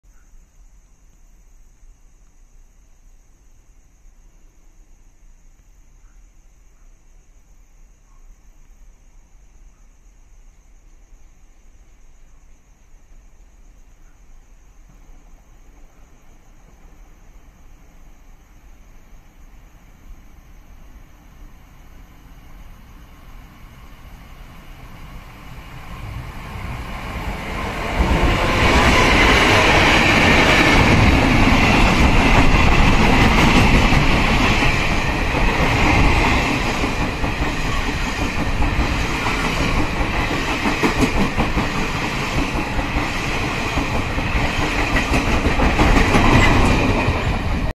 2020.03.03 7215次夜間迴送列車改點後第二拍
23:08分六塊厝通過
電本E228-機次位R137-敞(鋼輪)-3篷-無階自莒35FPK11504-守3AK2526-無階PP*7-守3CK2008

※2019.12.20改點後7215、7216電本編號不同且固定E+R配置
※7215=新左營迴送潮州基地
※今天的貨料不含E+R 為7201貨料(貨牌潮州基地)